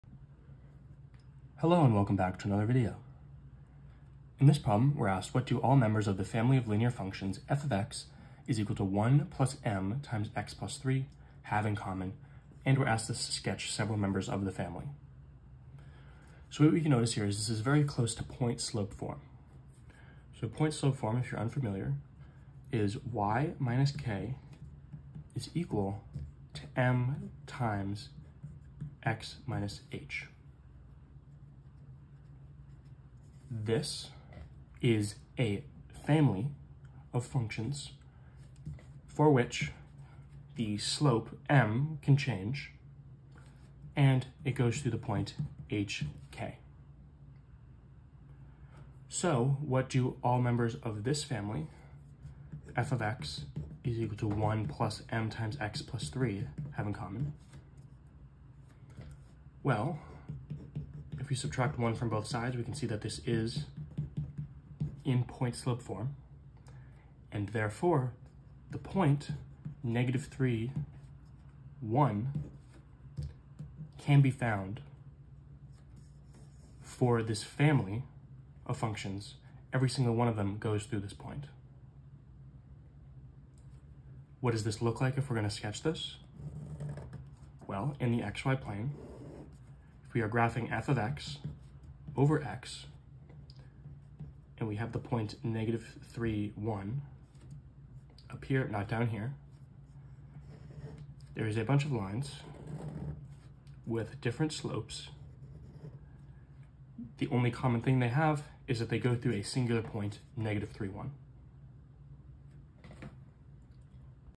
8. What do all members of the family of linear functions f(x)=1+m(x+3) have in common? Sketch...
8. What do all members of the family of linear functions f(x)=1+m(x+3) have in common? Sketch several members of the family.